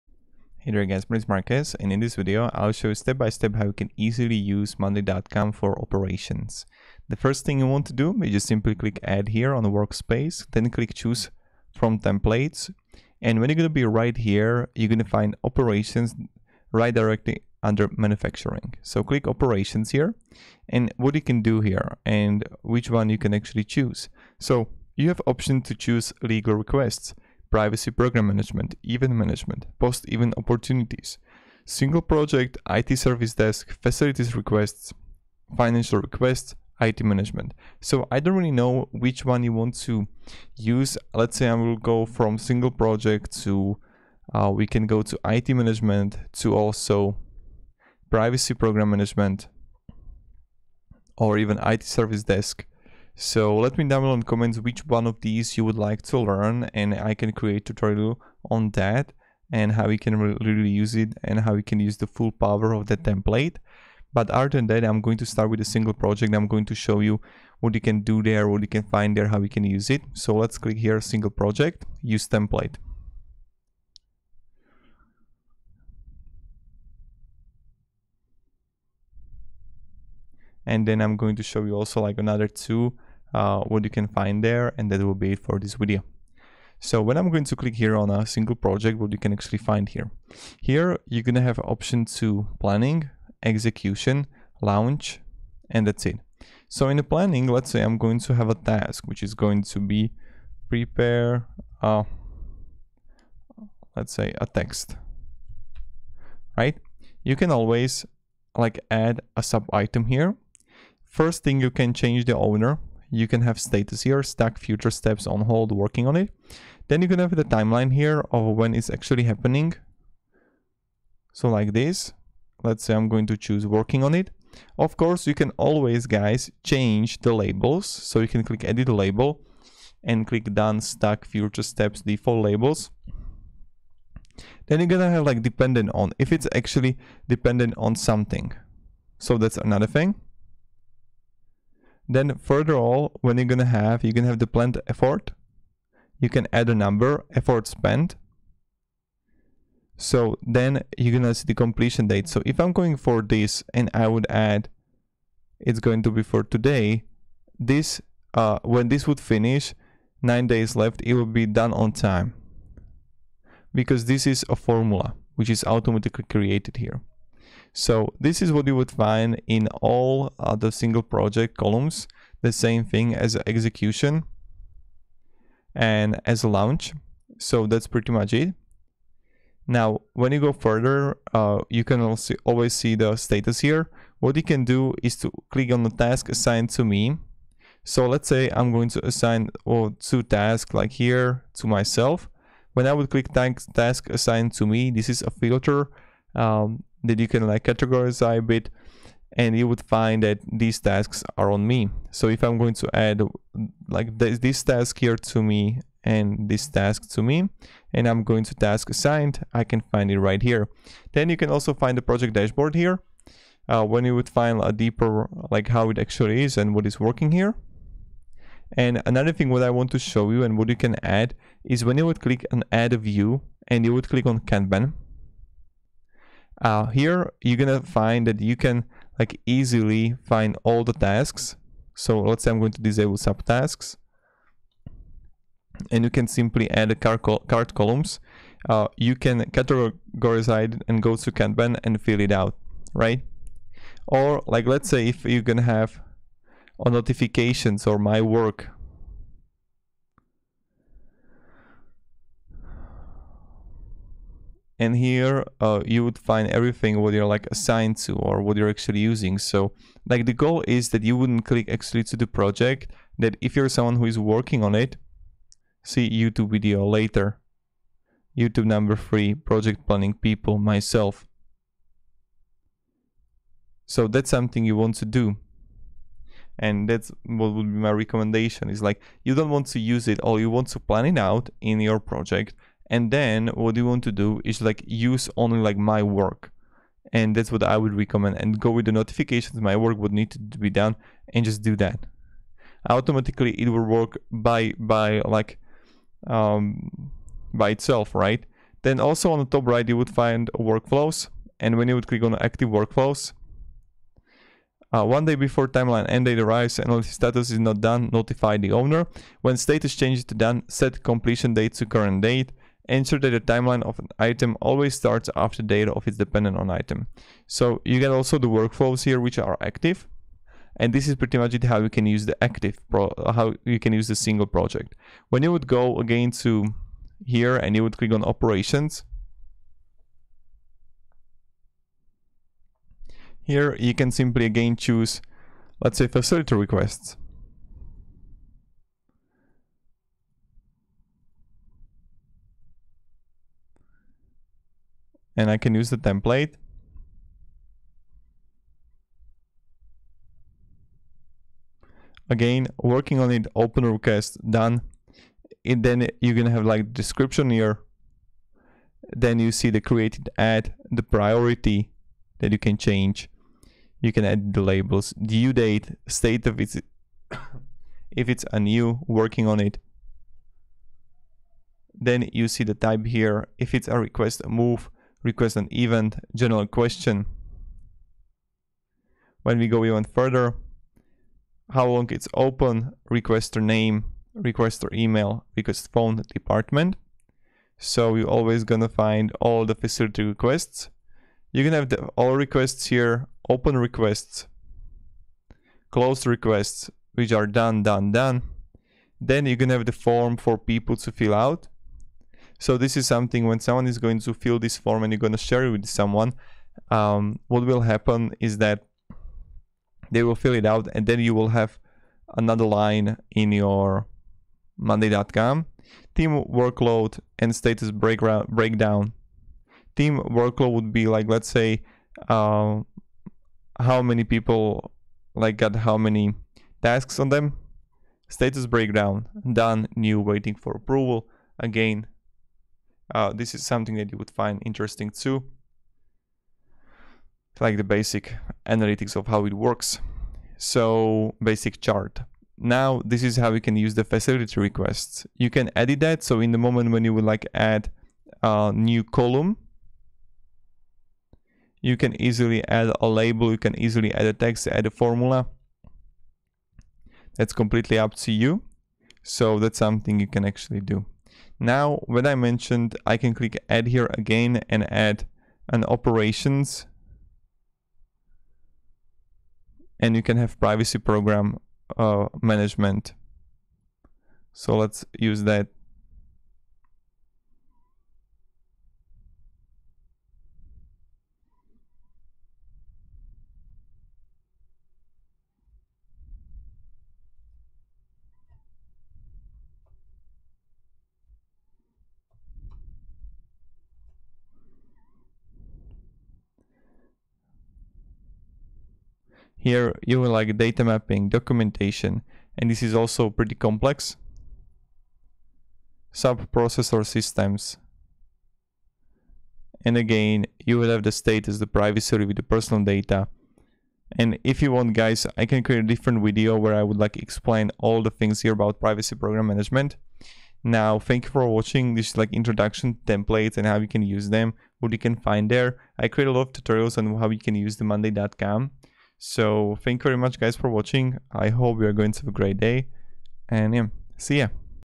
Monday.com For Operations - How To Use Monday.com For Operations
‏‏‎ ‎ In this video you will find out Monday.com For Operations - How To Use Monday.com For Operations